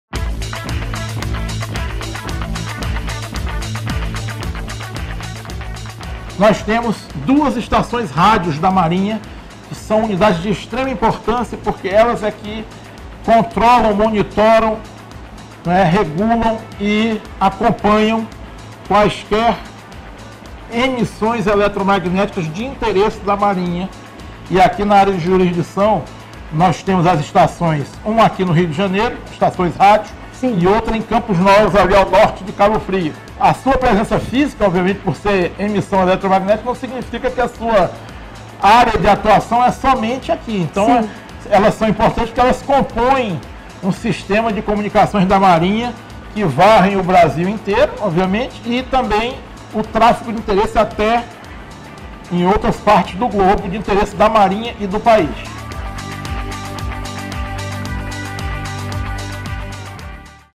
05/19 Estações Rádio da Marinha no 1º Distrito Naval - Vice-Almirante Flávio Augusto Viana ROCHA
Estações Rádio da Marinha no 1º Distrito Naval - por Vice-Almirante Flávio Augusto Viana ROCHA -  Comandante do Distrito

***Publicado em Maio de 2019***

Nós temos duas estações rádios da Marinha que são unidades de extrema importância porque elas é que controlam, monitoram, regulam e acompanham quaisquer emissões eletromagnéticas de interesse da Marinha. E, aqui na área de jurisdição, nós temos as estações, uma aqui no Rio de Janeiro, estações rádio, e outra em Campos Novos, ali ao norte de Cabo Frio. A sua presença física, obviamente, por ser emissão eletromagnética, não significa que a sua área de atuação é somente aqui. Então, elas são importantes porque elas compõem um sistema de comunicações da Marinha que varre o Brasil inteiro, obviamente, e também o tráfego de interesse até em outras partes do globo de interesse da Marinha e do país.